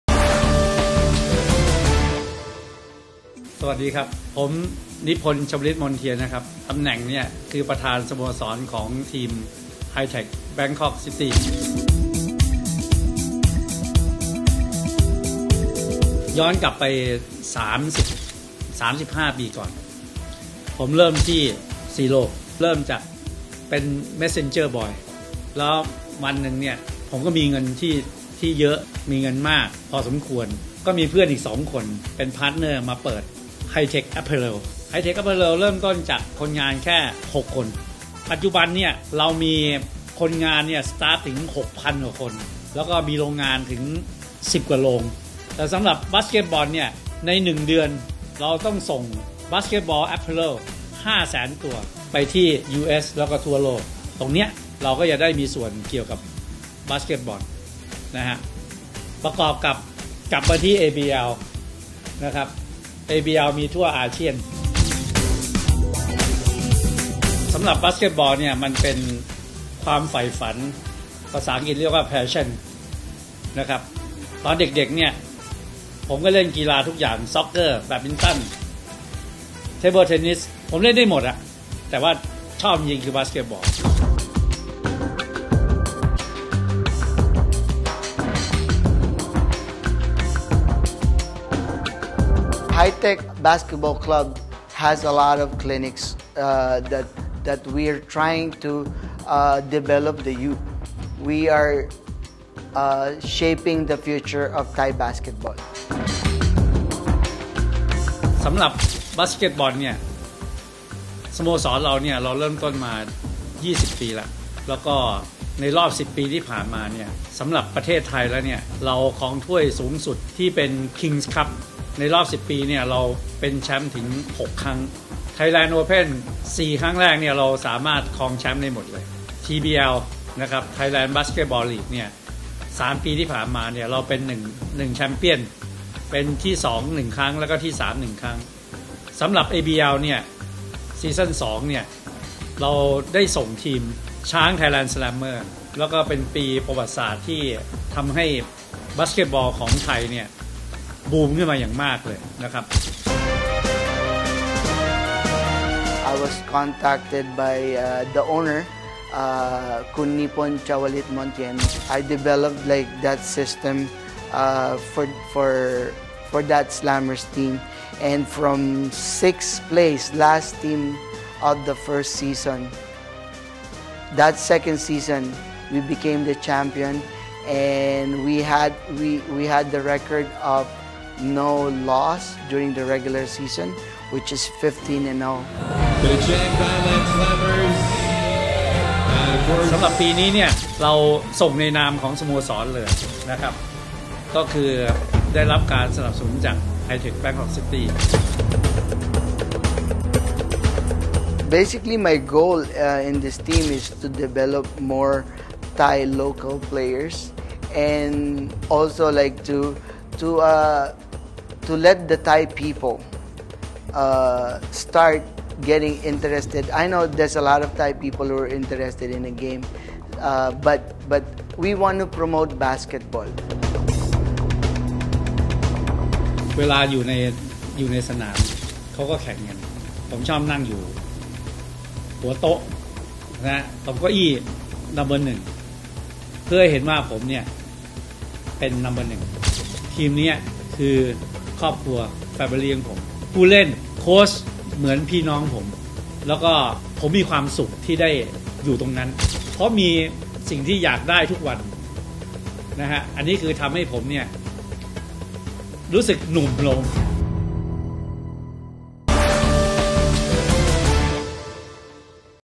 Team Profiles: Hi-Tech Bangkok City
Hi-Tech Bangkok City's owner Nipondh Chawalitmontien and coach Raha Mortel give us a brief history of the team and their expectations for the future.